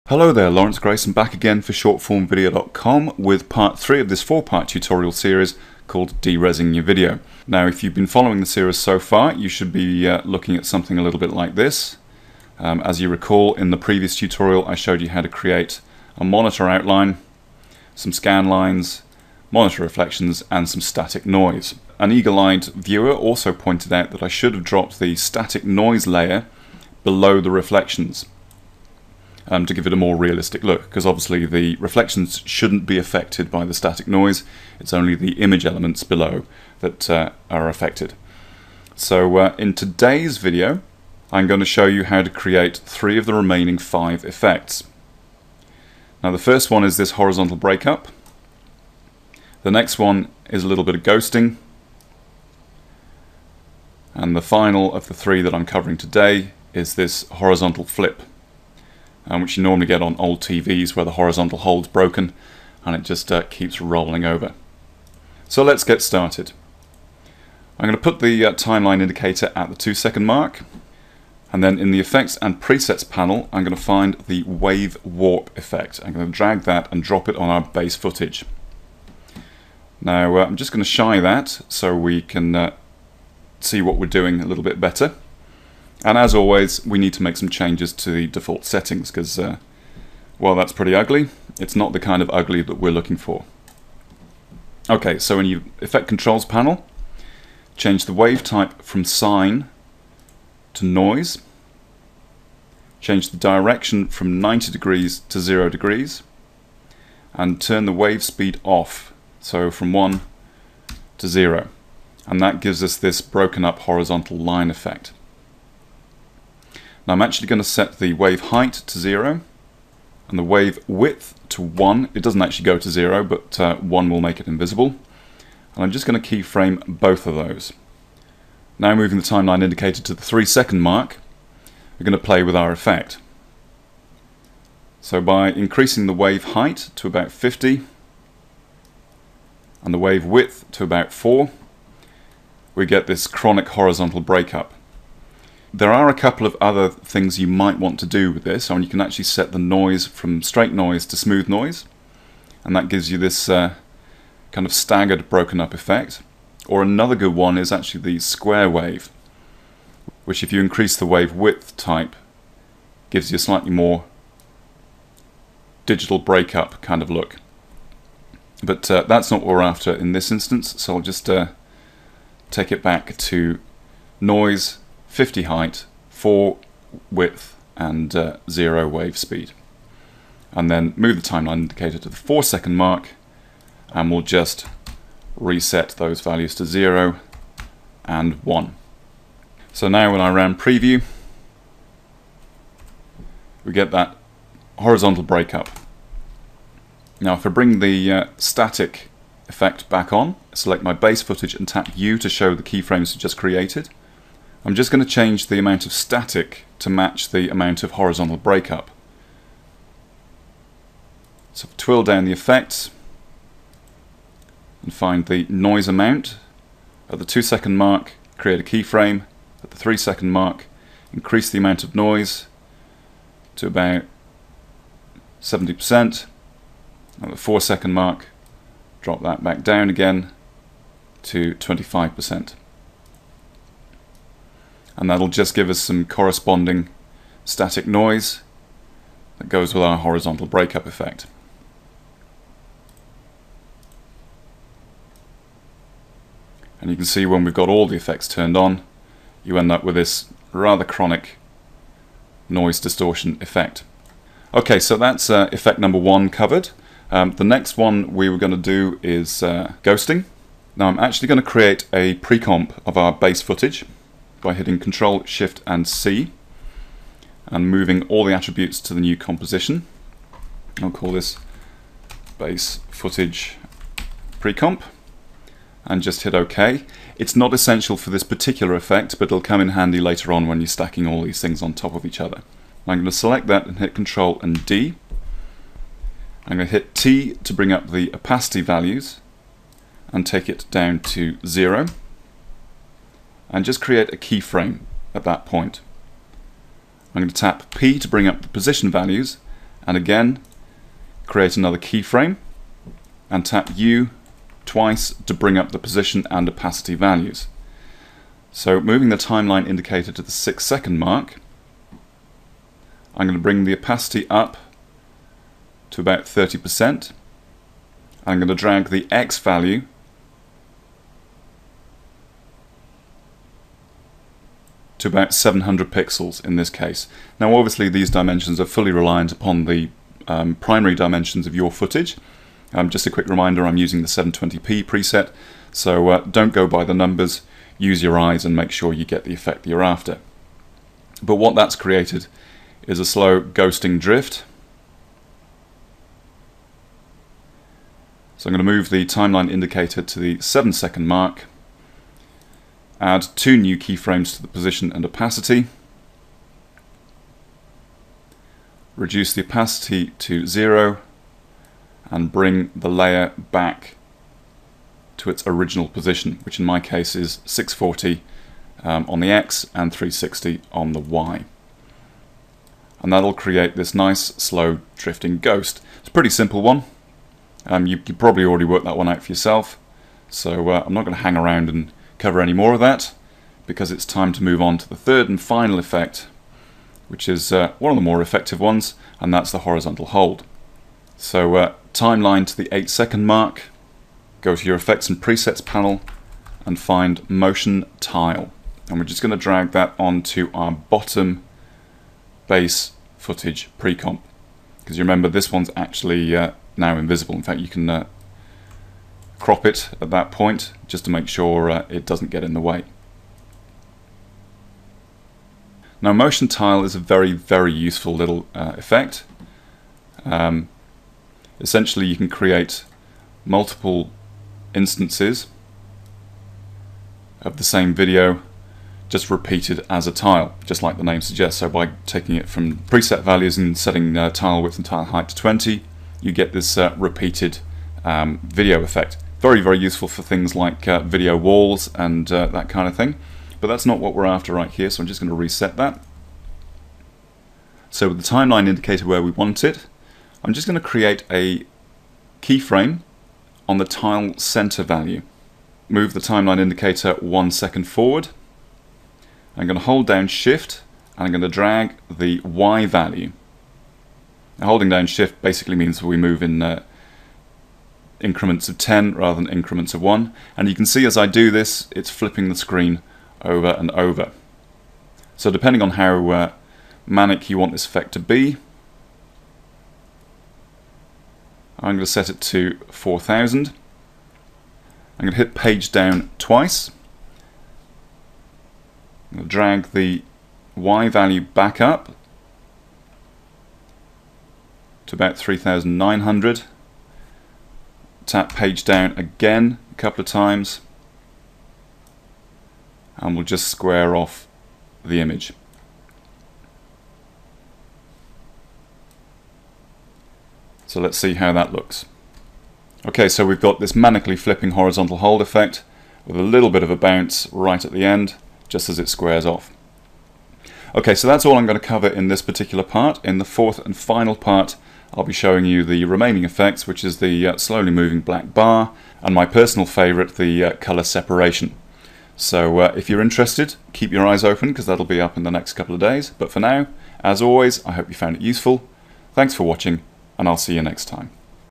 After Effects Tutorial - De-rezzing your video, pt3
Here's the third of this four-part tutorial series showing you some techniques to give your video a grunge, TV interference look. This part covers three of the remaining five effects, namely rolling picture (horizontal hold), severe noise and ghosting. The fourth and final part will be out shortly, and will cover the final effects - scrolling black bar and colour channel separation.